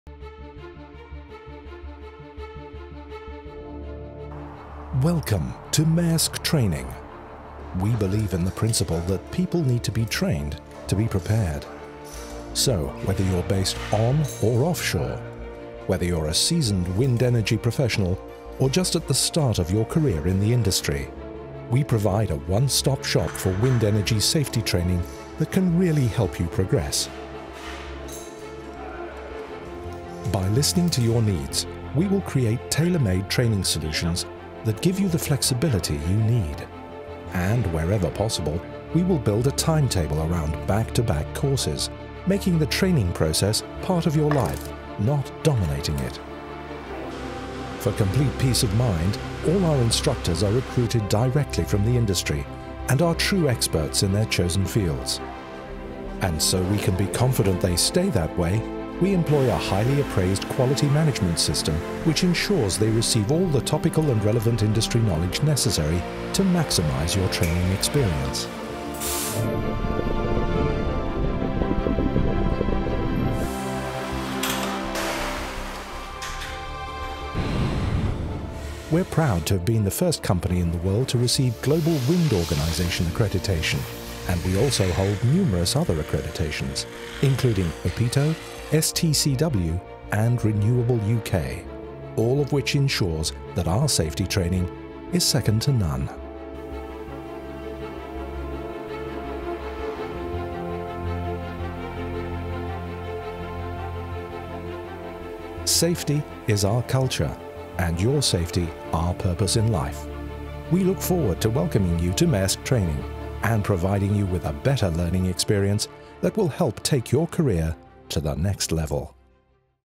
Training for the wind energy industry
Maersk Training delivers the safety courses required by major companies in the wind power industry, supplemented by technical training for several operators.

We hold course accreditations from industry leaders like Siemens Wind Power and Vestas. This makes us a one-stop shop for the wind industry, and we plan our courses for just-in-time training.

We can deliver the courses in one back-to-back session when our clients need it, as well as plan to the specific demands of each client. We say flexibility without compromise.

Brand new facilities

Our bases are at the wind industry hubs of Denmark and the UK. We utilise brand new facilities to create life-like learning experiences. Training can also be delivered at other training centres, including courses required for working in the UK.

First GWO approval for Basic Safety.

Maersk Training is the first training centre in the world to be approved by the Global Wind Organisation (GWO) for all five modules of the Basic Safety Training standard:
o    First Aid
o    Manual Handling
o    Fire Awareness,
o    Working at Heights
o    Sea Survival
The assessment has been carried out by Det Norske Veritas (DNV).